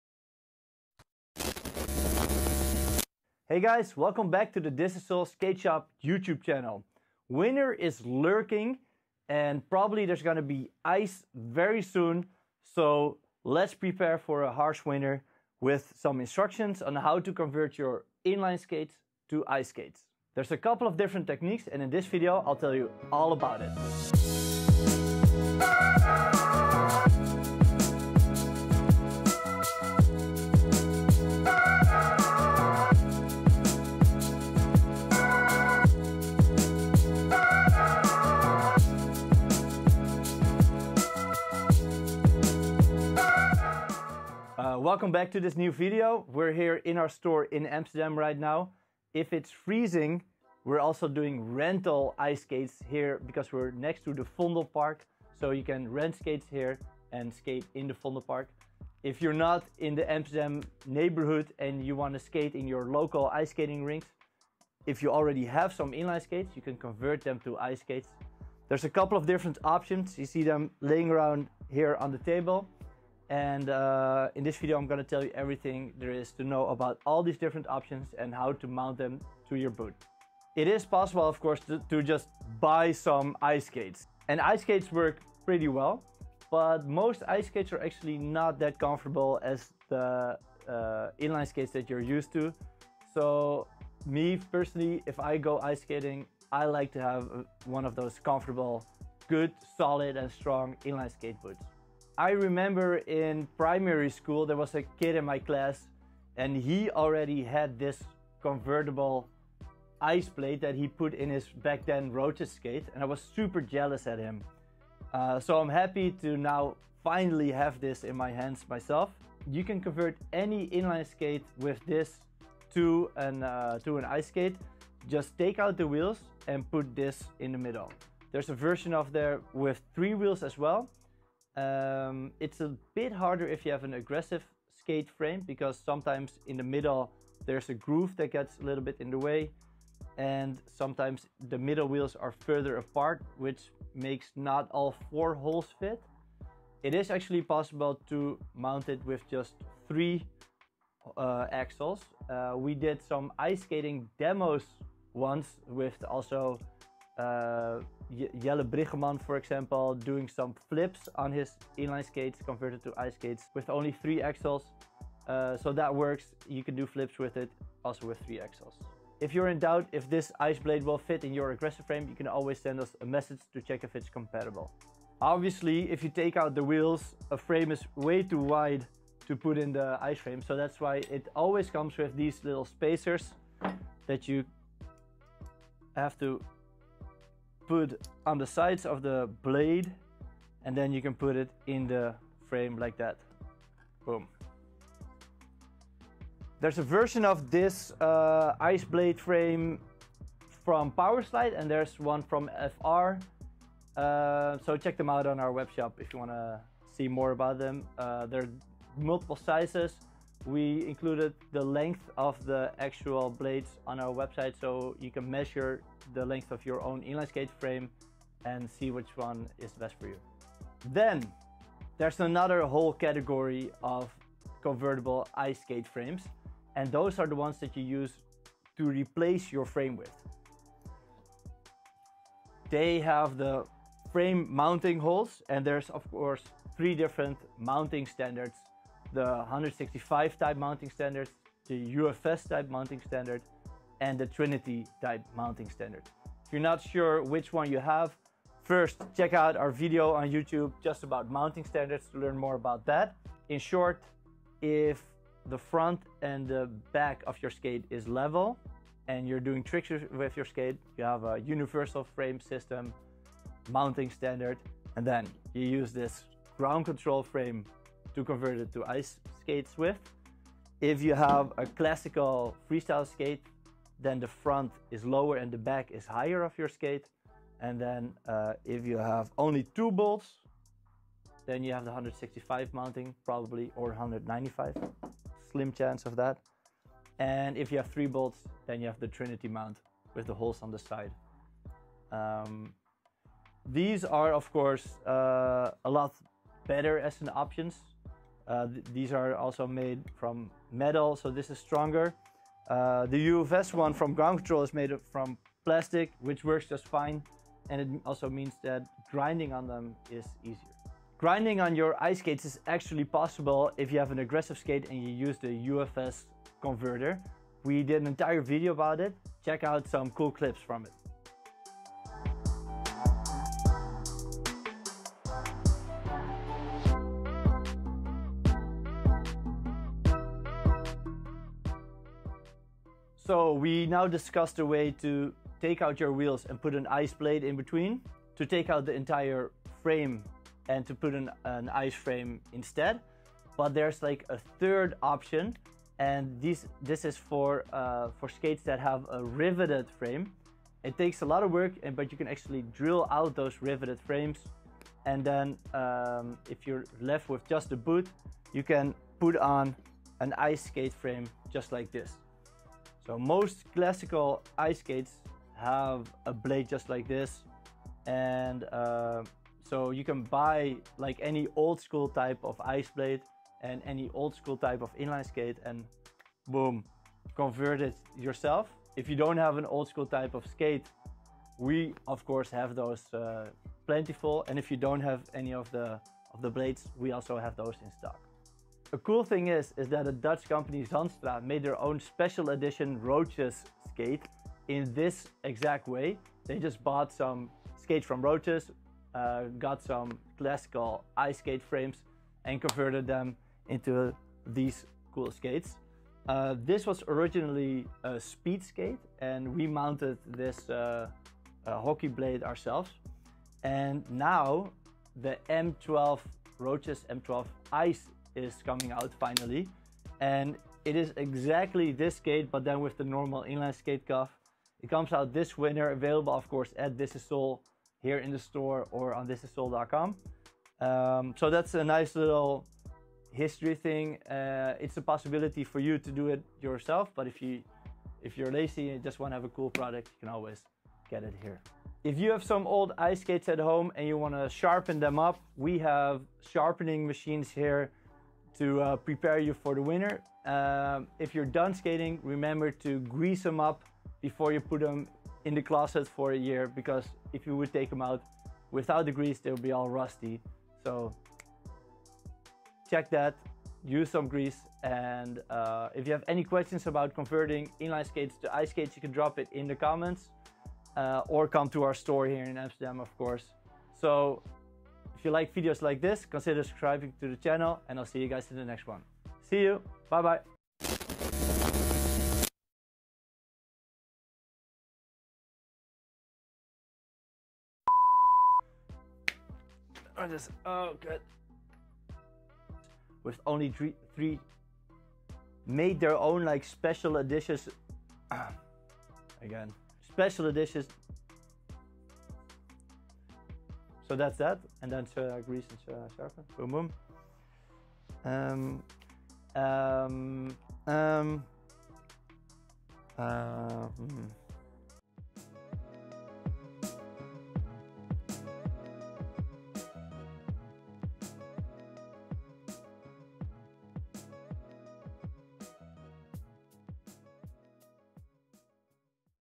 HOW TO CONVERT INLINE SKATES INTO ICE SKATES
In this Video Ivo explains how to convert your Inline Skates into Ice Skates, there are a couple of different ways to do this and Ivo guides you through all of them.

Do you want to learn to skate? Then take some lessons